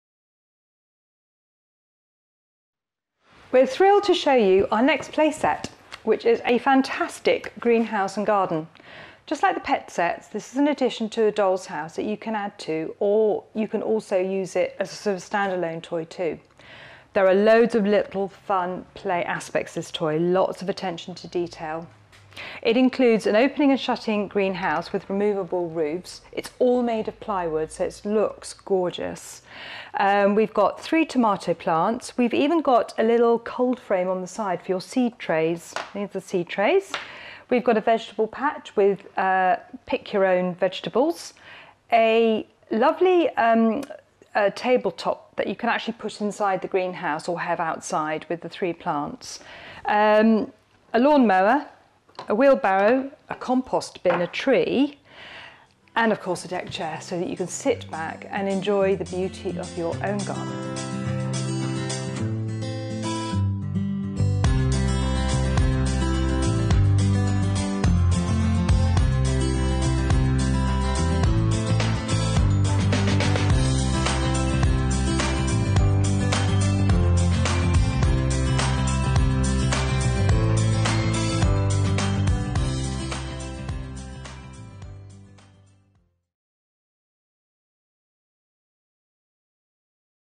Drewniana szklarnia Greenhouse and Garden Set Tender Leaf Toys
Wysokiej jakości drewniane domki i szklarnie dla lalek od Tender Leaf Toys. Drewniana szklarnia  Greenhouse and Garden Set z otwieranym dachem i akcesoriami już od 3 roku życia.
TL8371